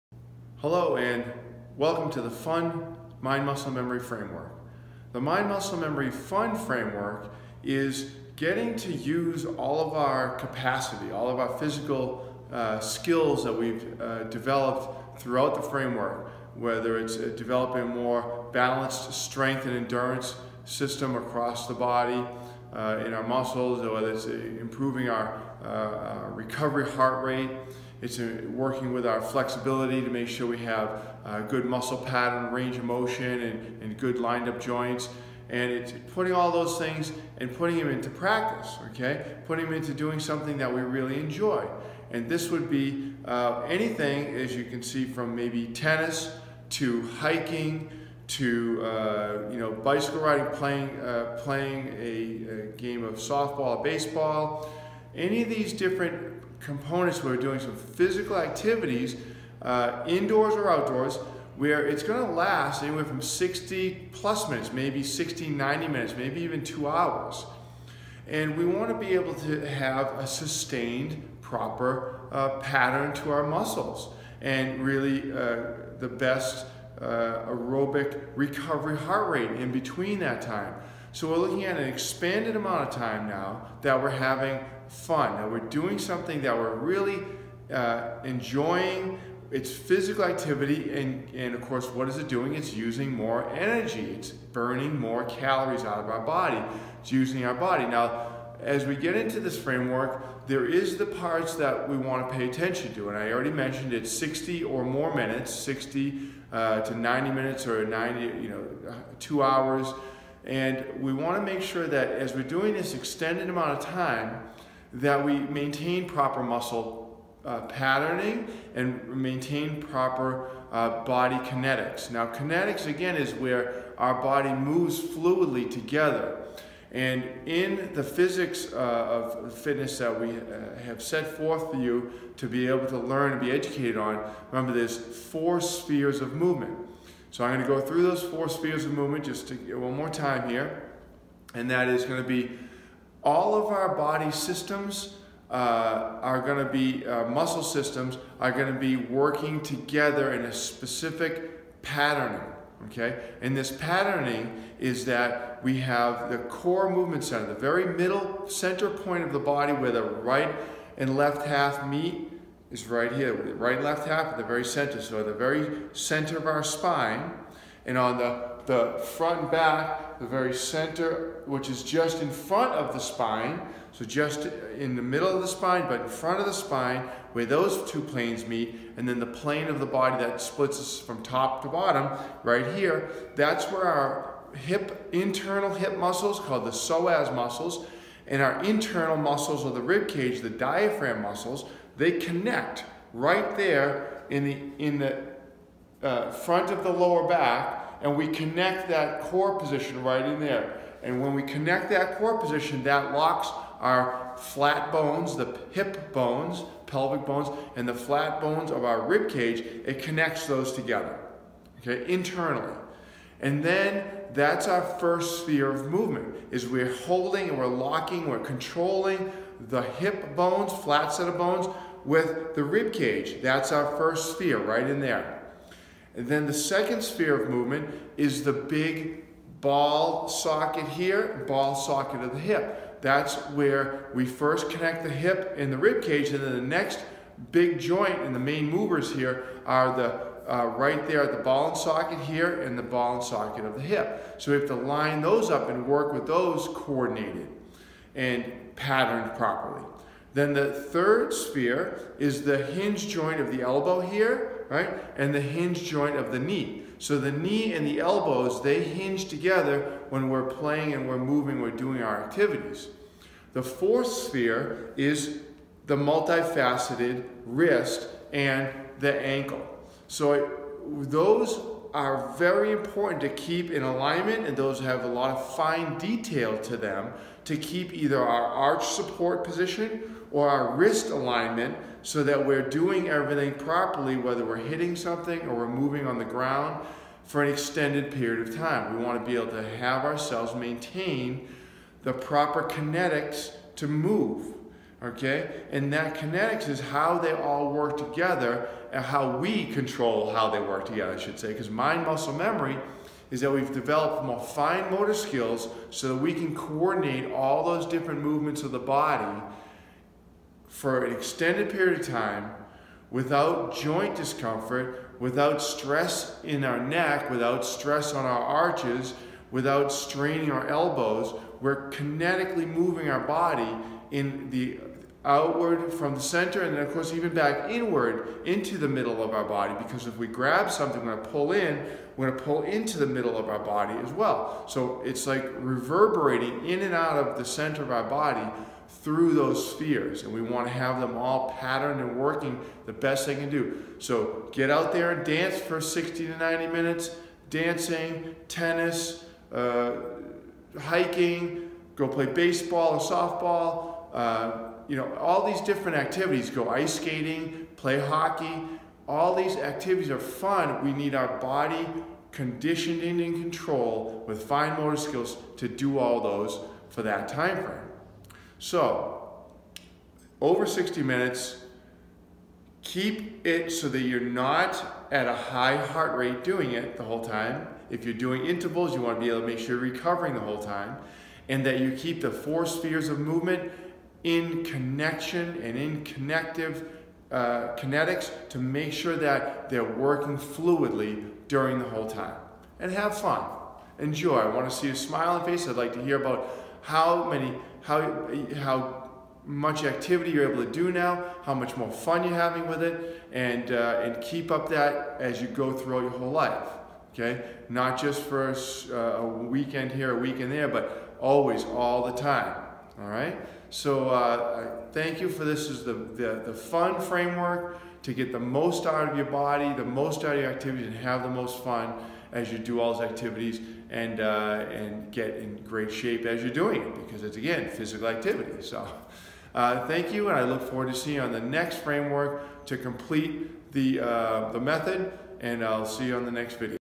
Fun for fitness and sports performance for all ages
The fun framework is the strategy to elevate our game to our best skills and increase output.

Mind Muscle Memory Inc Headquarters